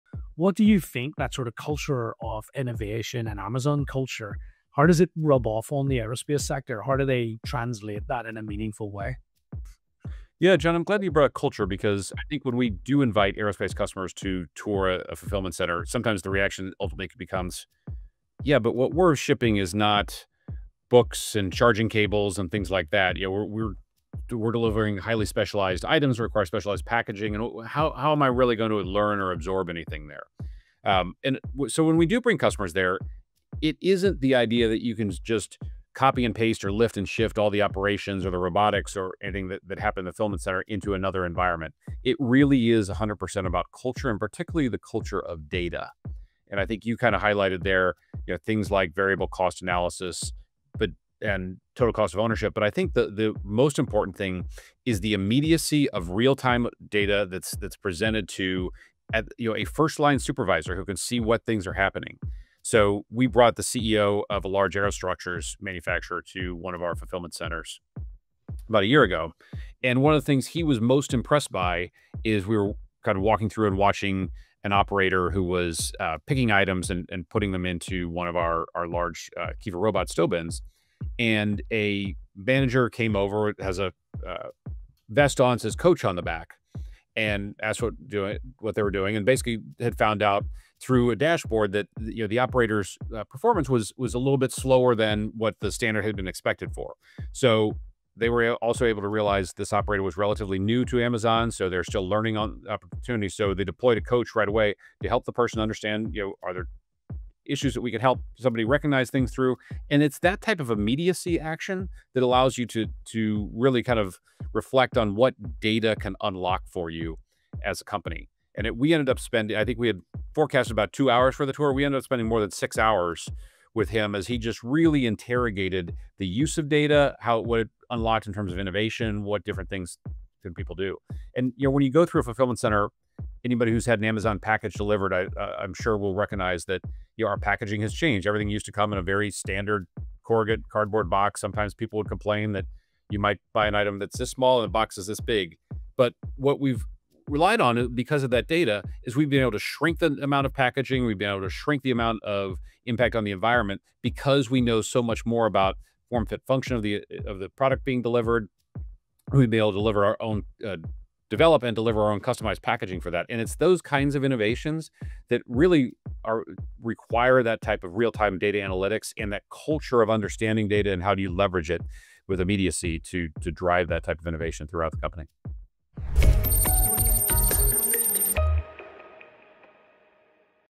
Bringing Amazon’s Innovation DNA into Aerospace
What does Amazon’s culture of innovation offer aerospace? Tim Murnin explores how mechanisms like “working backwards” and decentralised teams are reshaping how aerospace thinks.